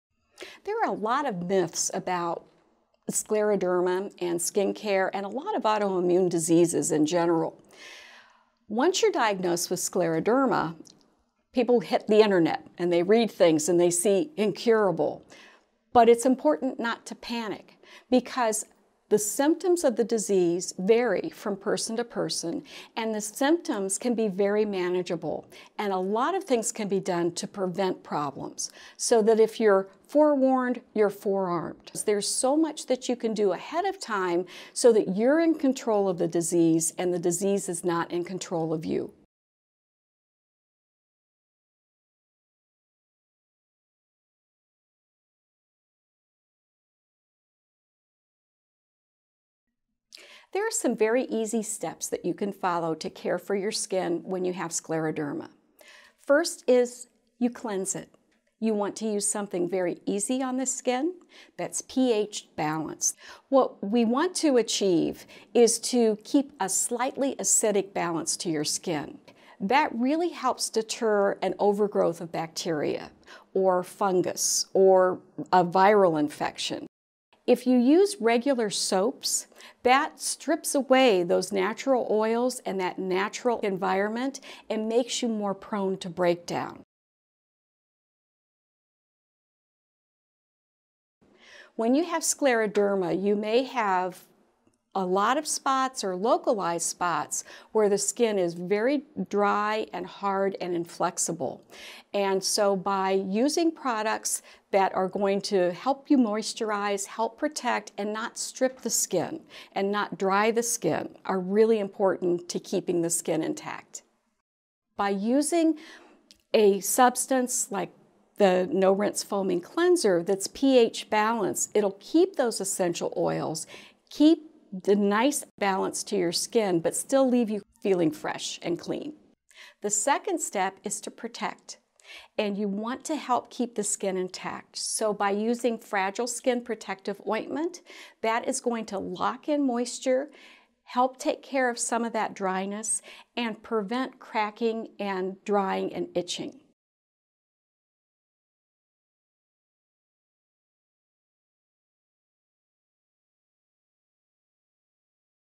Scleroderma skin care 1 of 4: How to care for your skin
Learn how to care for skin affected by scleroderma with expert tips from Paula Erwin-Toth, former Director of WOC Nursing Education at Cleveland Clinic.